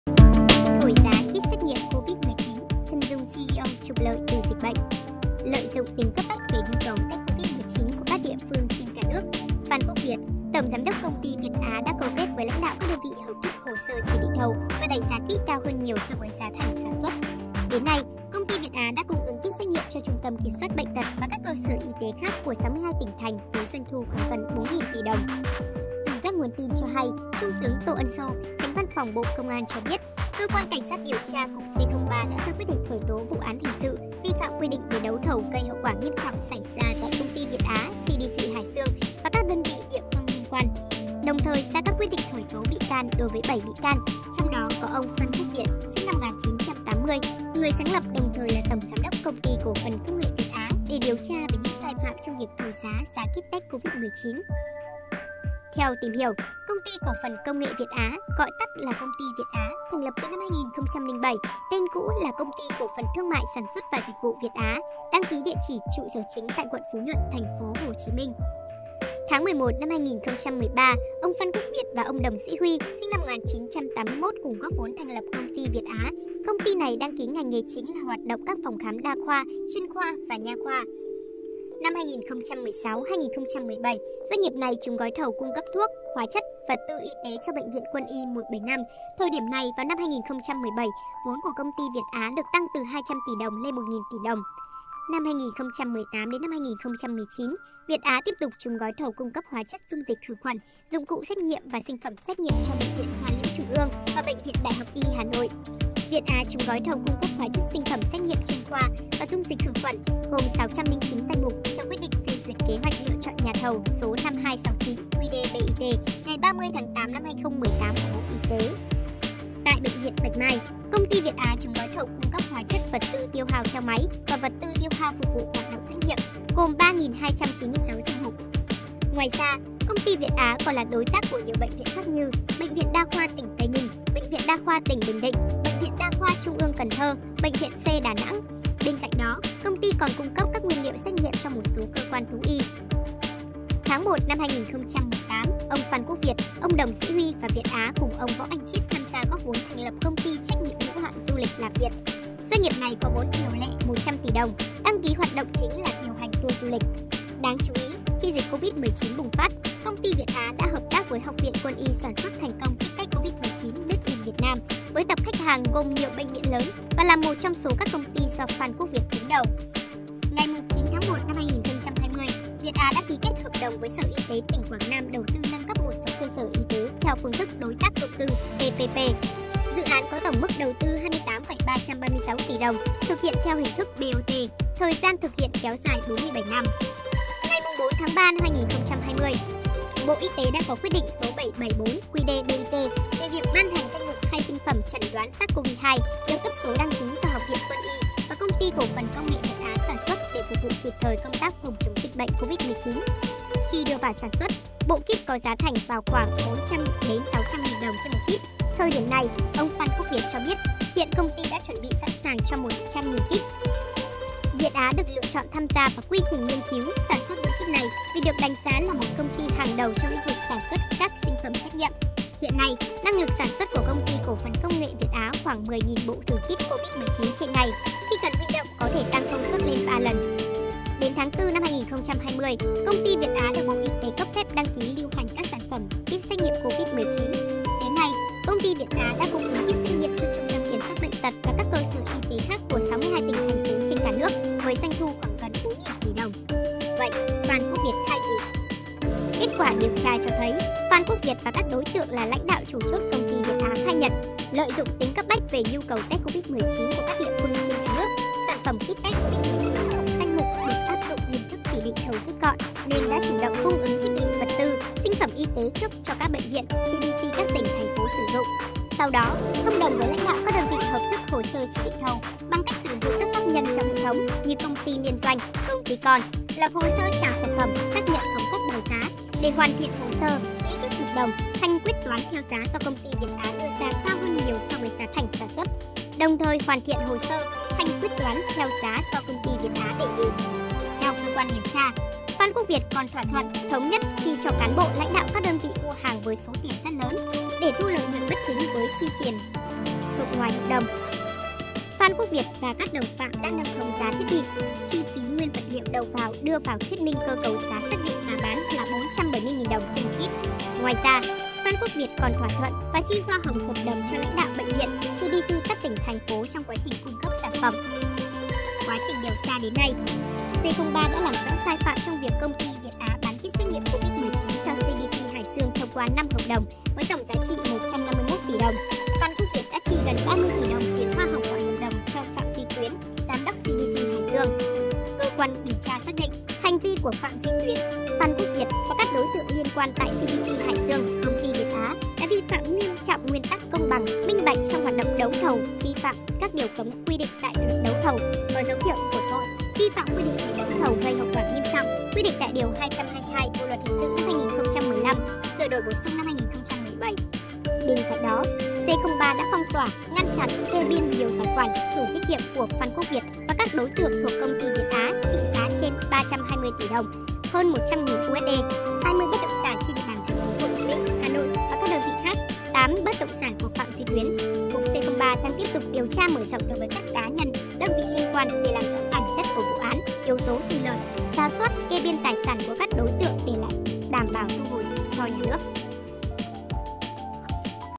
“THỔI GIÁ” KIT XÉT NGHIỆM COVID-19: CHÂN DUNG CEO TRỤC LỢI TỪ DỊCH BỆNH
Lợi dụng tính cấp bách về nhu cầu test COVID-19 của các địa phương trên cả nước, Phan Quốc Việt, Tổng Giám đốc Công ty Việt Á đã câu kết với lãnh đạo các đơn vị hợp thức hồ sơ chỉ định thầu và đẩy giá Kit cao hơn nhiều so với giá thành sản xuất.
Đến nay, Công ty Việt Á đã cung ứng Kit xét nghiệm cho Trung tâm kiểm soát bệnh tật và các cơ sở y tế khác của 62 tỉnh, thành với doanh thu khoảng gần 4.000 tỷ đồng.
Đồng hành cùng Tre Làng TVi tại